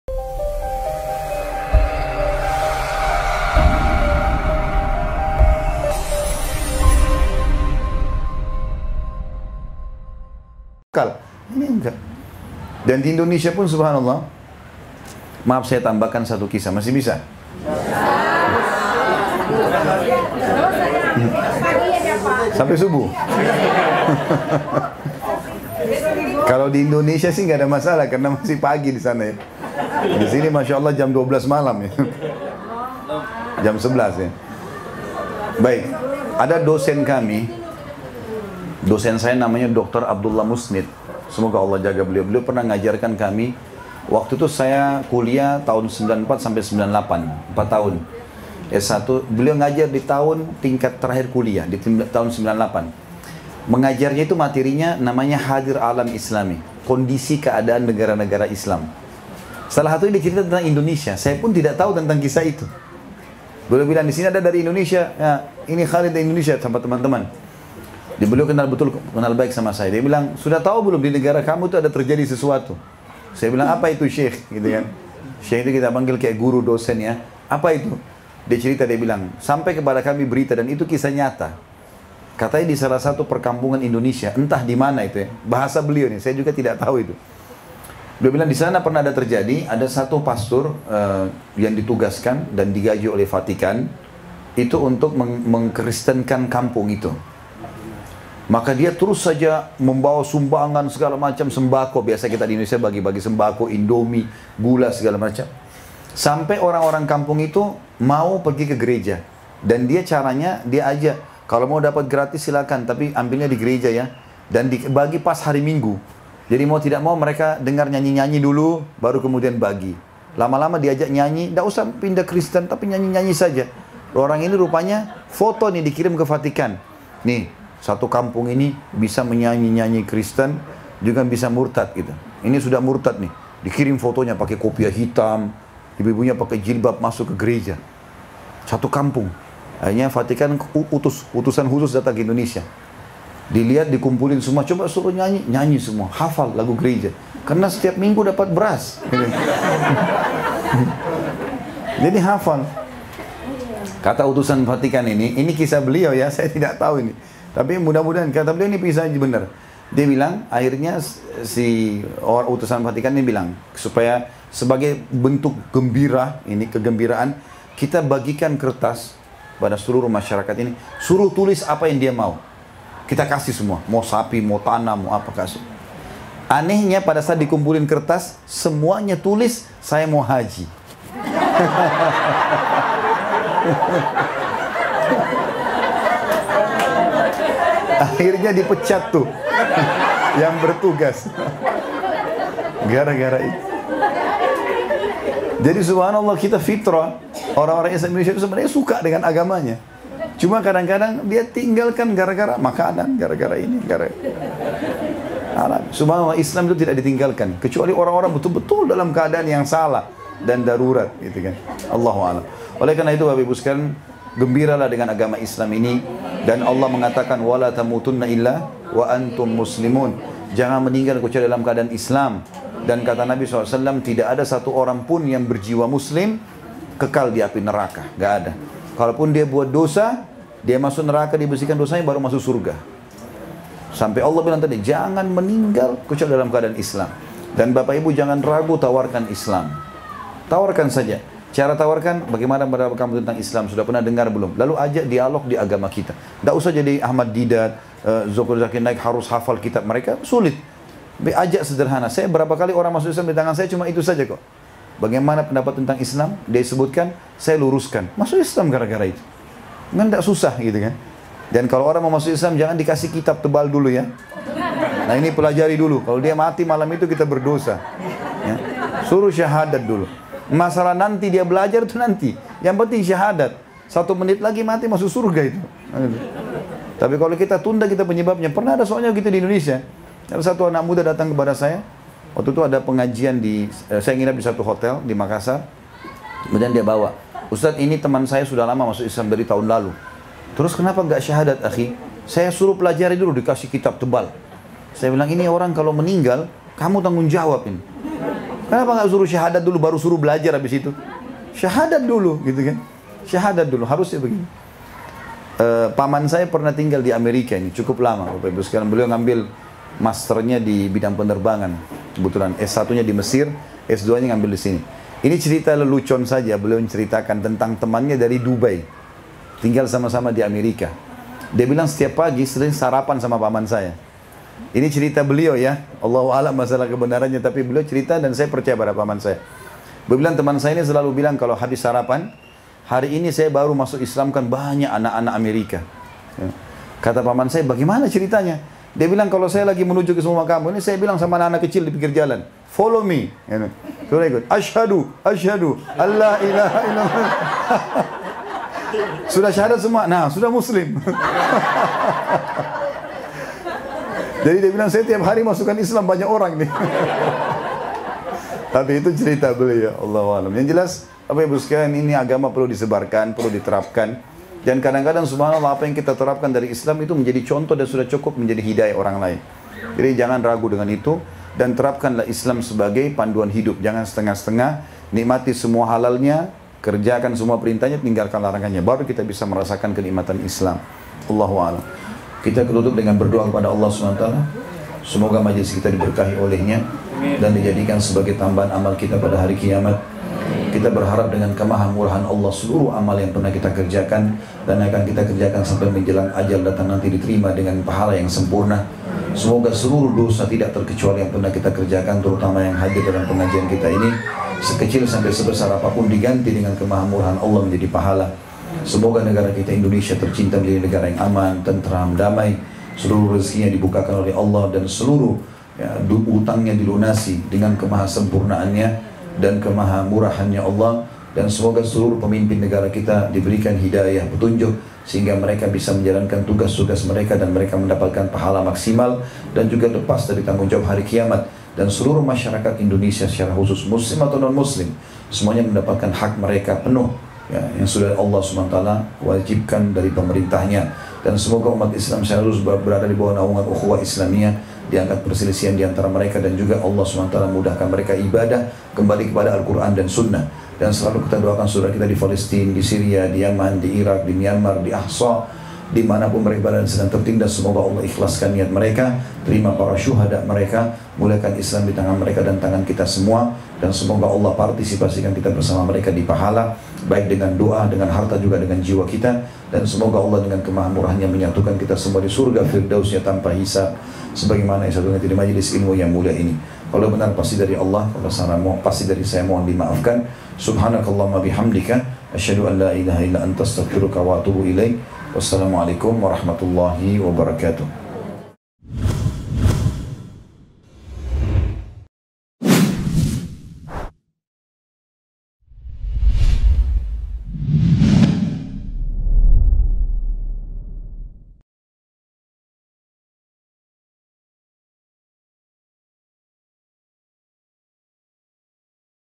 KISAH LUCU !!! ORANG INDONESIA minta haji ke pendeta di Gereja || Ustadz Dr. Khalid Basalamah, MA
Imam Muslim meriwayatkan dalam kitab Shahih-nya, hadits dari sahabat Uqbah bin ‘Amr bin Tsa’labah radhiallahu’anhu, bahwa Rasulullah Shallallahu’alaihi Wasallam bersabda:

Kajian ini dari ustadz yang bermanhaj salaf, berdasarkan Al Quran dan Hadist dengan pemahaman para Salafus Shalih (Sahabat, Tabiin, Tabiut-Tabiin)